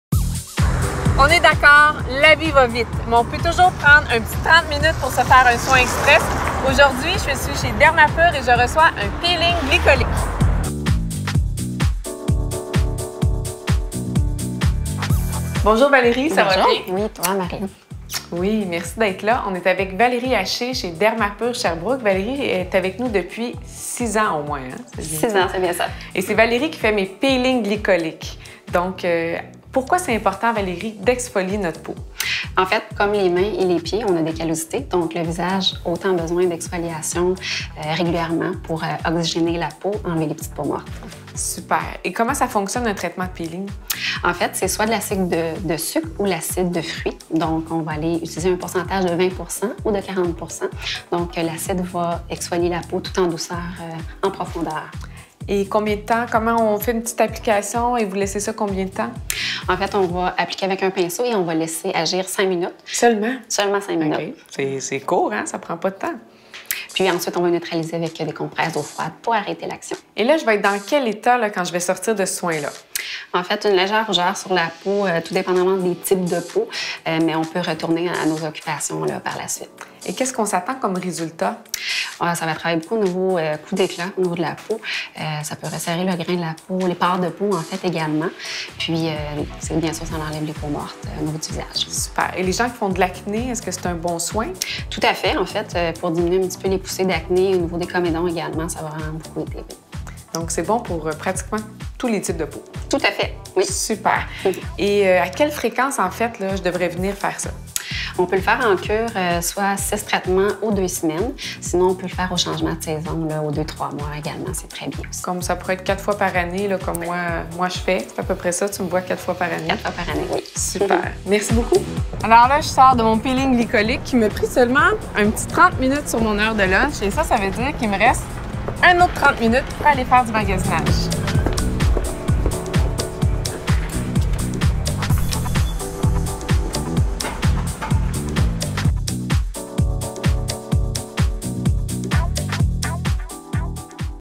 Glycolic Peeling - Gym for your skin
Not having the time is no longer an excuse: a glycolic peel is a fast and effective anti-aging exfoliation that tightens pores and renews skin!